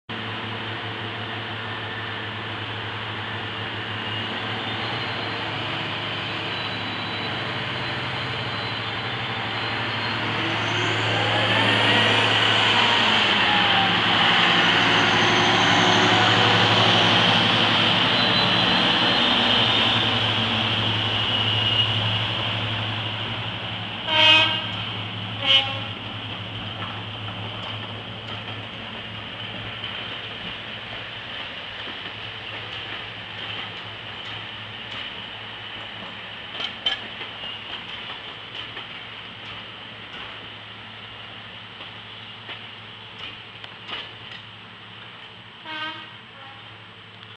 55022 19th Jan 2008 ELR running day part 13
Royal Scots Grey departs witrh the last northbound train of the day, the 17.20 to Ramsbottom